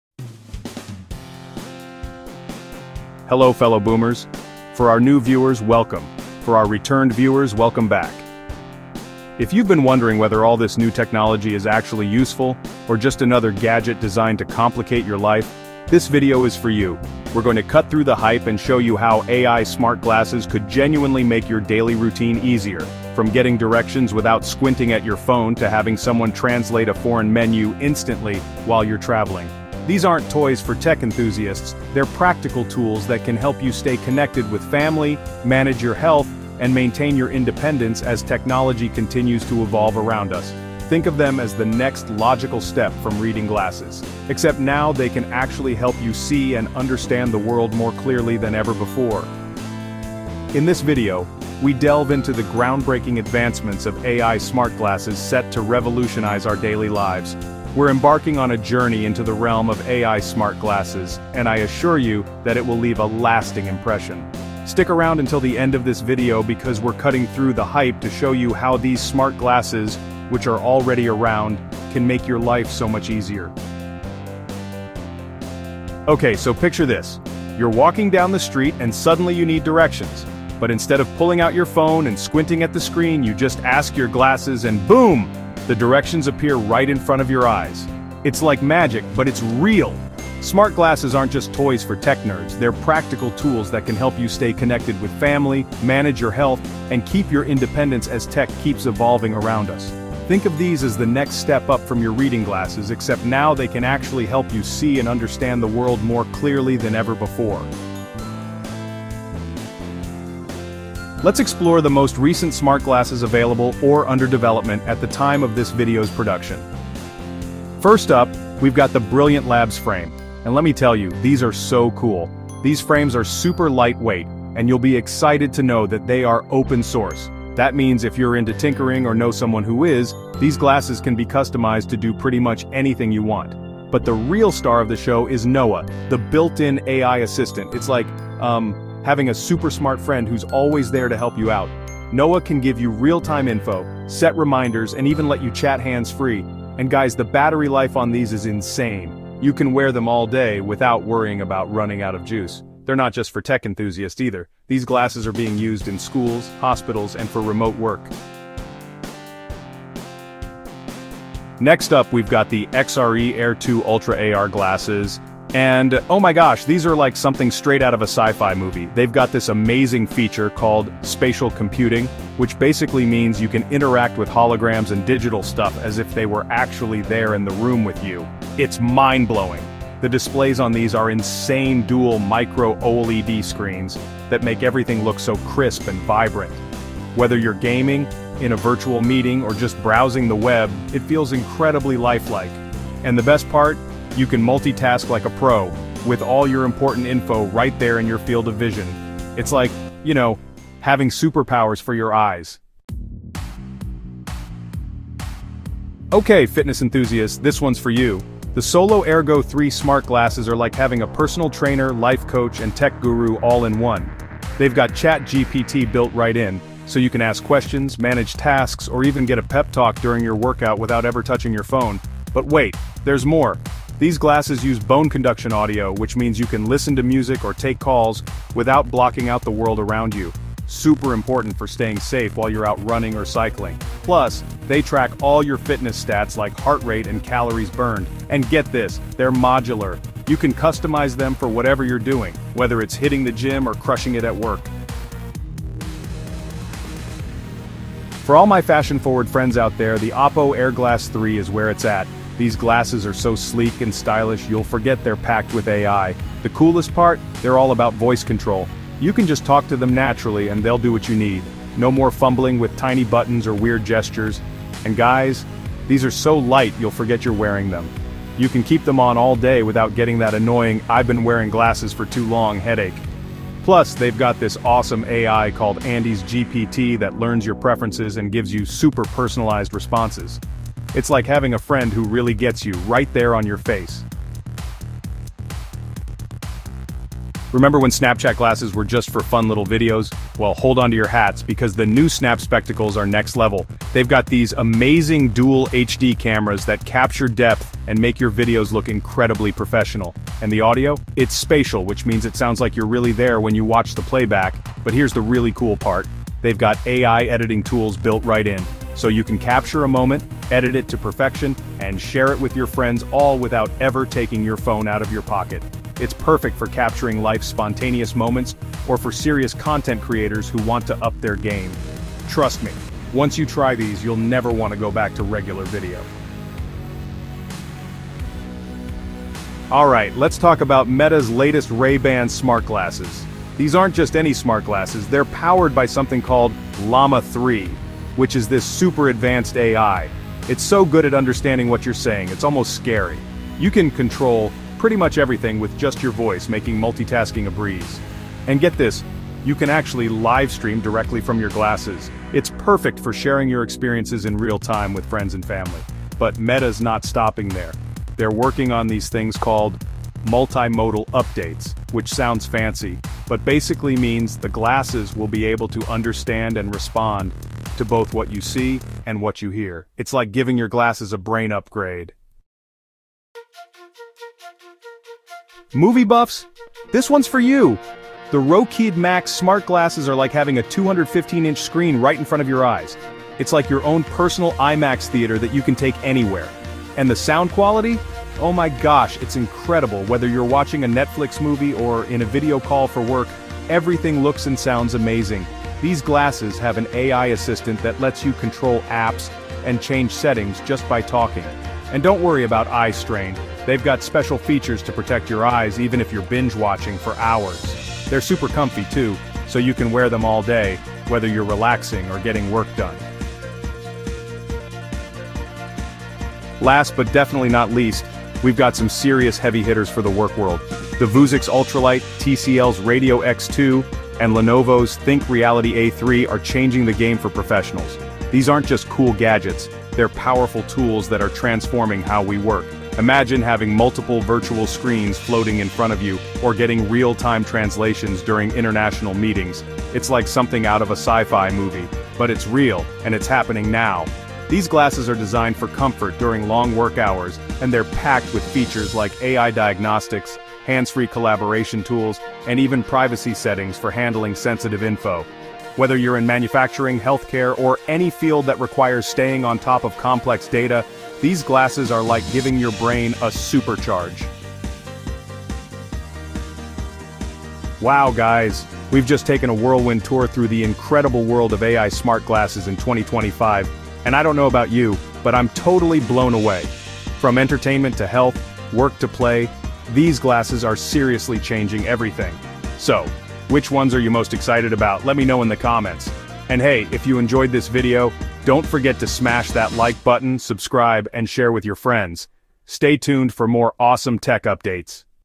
Make Everyday Tasks EASIER with AI Smart Glasses
Explore how AI glasses can genuinely make your daily routine easier. From getting directions to translating menus instantly, the possibilities of augmented reality are endless. These glasses represent practical future technology that provides useful applications of artificial intelligence.

Discover the future with our in-depth guide to AI Smart Glasses 2025, designed especially for boomers! This video cuts through the tech jargon to reveal how smart glasses can simplify your life—from effortless navigation to instant language translation. We explore top models like Brilliant Labs' Frame with customizable AI assistant Noah, XRE Air2 Ultra’s stunning AR visuals, Solo Airgo 3’s fitness-focused features with ChatGPT, and Meta’s Ray-Ban powered by Llama 3. Whether for productivity, entertainment, or staying connected, these smart glasses offer stylish, practical solutions for aging gracefully in a tech-driven world. Watch until the end to find the perfect AI smart glasses tailored to your needs.